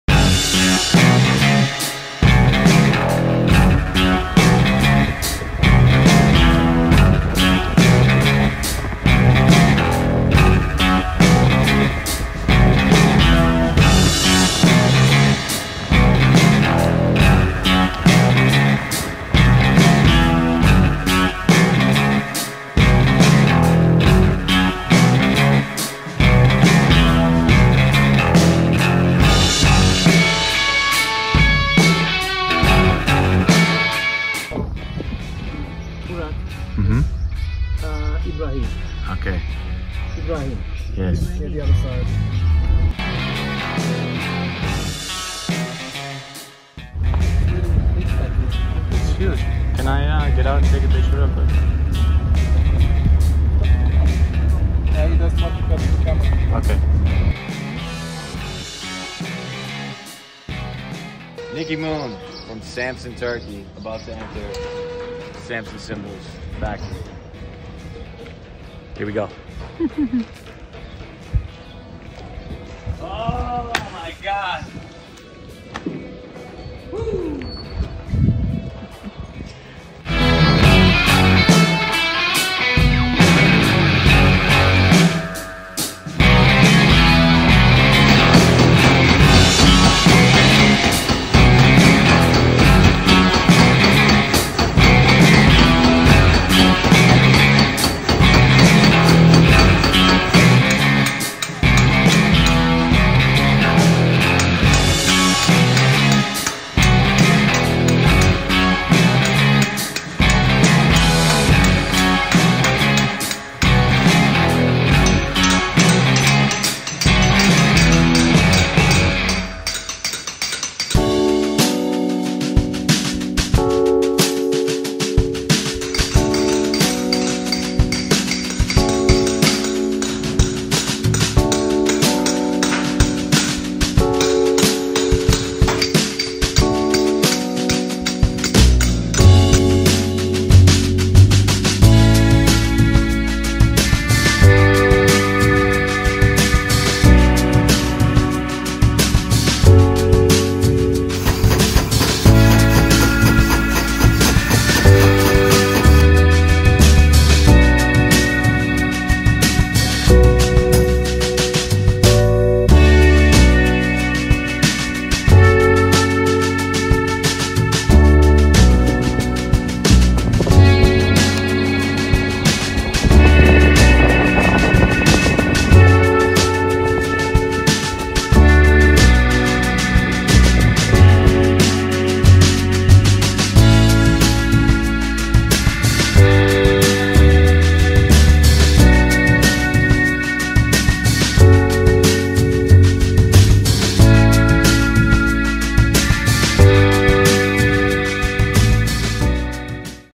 Cymbal Pilgrimage pt 1: Samsun, Turkey
NickyMoon made a cymbal pilgrimage earlier this summer to Samsun and Istanbul Turkey to connect with his heritage, pay homage to the masters and learn some new skills.

Stomp
Wise